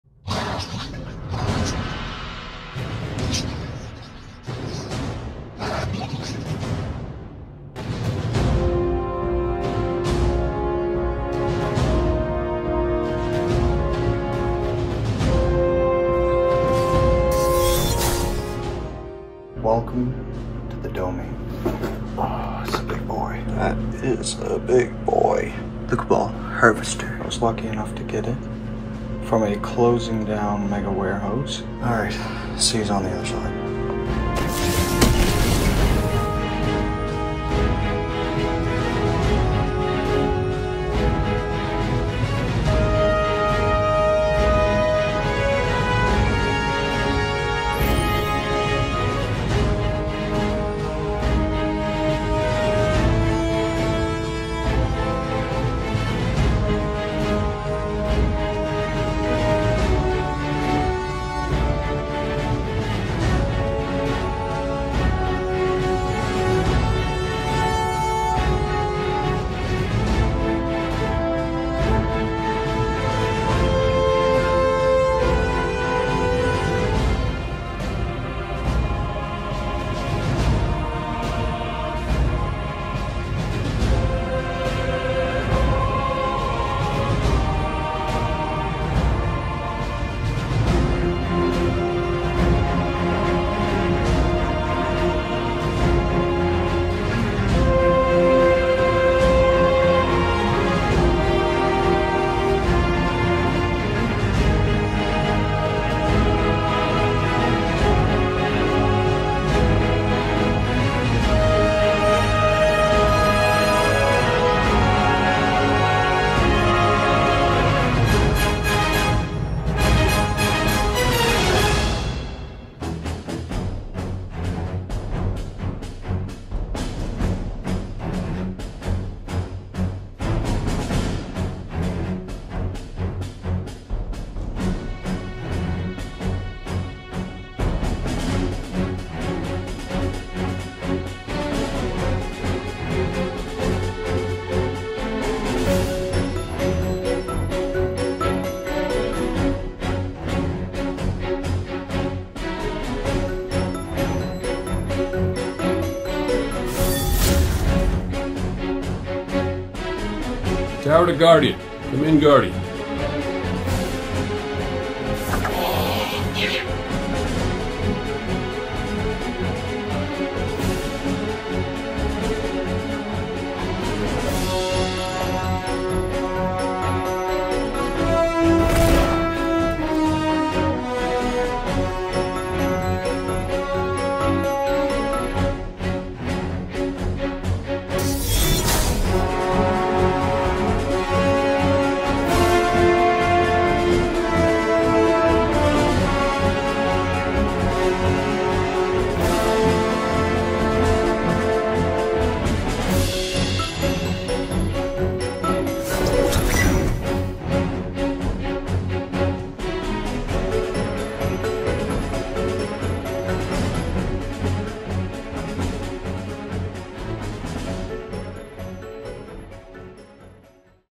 Destiny Cabal Harvester Dropship is AWESOME! Signature Series Build!
Cayde-6 Takes on the Red Legion with This Mega Construx Signature Series dropship!

The hordes of the Red Legion are ready to deploy from their massive Harvester Dropship. But while the Cabal foes advance, Cayde-6 waits for the perfect moment to strike! With authentic details inspired by the new Destiny game, now you can build this hulking troop carrier piece by piece. Next, open the rear hatch and side doors to drop your Cabal troops into action. Then launch forward, and take the enemy by surprise as Cayde-6 unleashes a dual-wield attack with his Ace of Spades in hand!

Simon Key
11E Cherish Court
Discovery Bay
Hong Kong